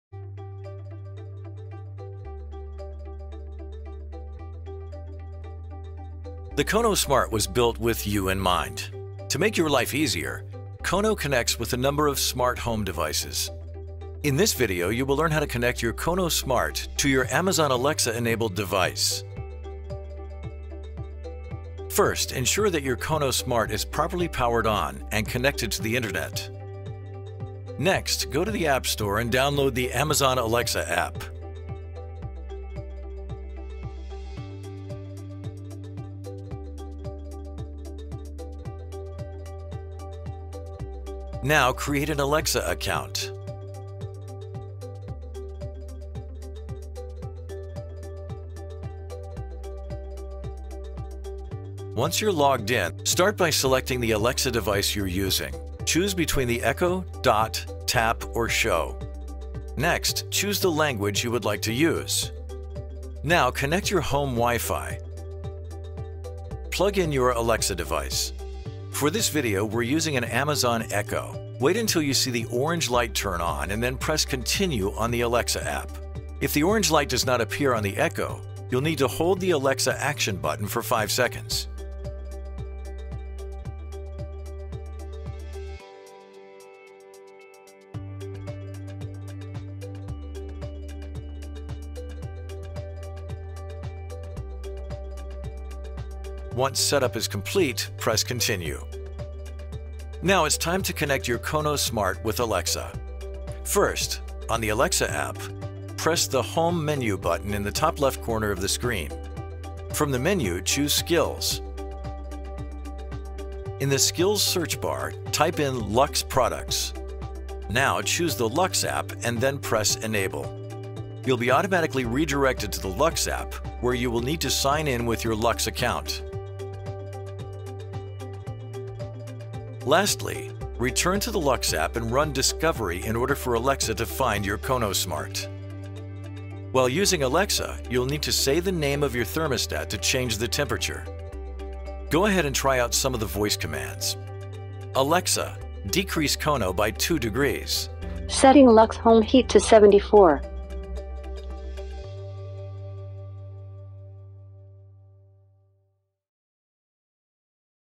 Connecting KONO Smart to an Alexa Enabled Device
The KONO Smart was built with you in mind. To make your life easier, KONO connects with a number of smart home devices. In this video, you will learn how to connect your Kono Smart to your Amazon Alexa enabled device.

First, ensure that your KONO Smart is properly powered on and connected to the Internet. Next, go to the app store and download the Amazon Alexa app.

Now, create an Alexa account. Once you are logged in, the screen will read “Begin Set Up”.

Start by selecting the Alexa device you are using. Choose between the Echo, Dot, Tap or Show.

Next, choose the language you would like to use.

Now, connect to your home WiFi.

Once connected, plug in your Alexa device. For this video, we are using an Amazon Echo. Wait until you see the orange light turn on and then press continue on the Alexa App. If the orange light does not appear on the Echo, you will need to hold the Alexa action button for five seconds. The app will then ask you to allow access for Alexa. Choose Allow. Once set up is complete, press continue.

Now it’s time to connect your KONO Smart with Alexa. First, on the Alexa app, press the home menu button in the top left corner of the screen. From the menu, choose skills. In the skills search bar, type in LUX Products. Now, choose the LUX App and then press Enable. You will be automatically redirected to the LUX App where you will need to sign in with your LUX account. Next, press Authorize to allow Alexa access to your LUX App. Lastly, return to the LUX App and run discovery in order for Alexa to find your KONO Smart. Now, name your thermostat. While using Alexa, you will need to say the name of your thermostat to change the temperature.

Go ahead and try out some of the voice commands. Say Alexa, set KONO to 75 degrees. Alexa, decrease KONO by 2 degrees. Alexa, increase KONO by 2 degrees. And now you’re ready to go.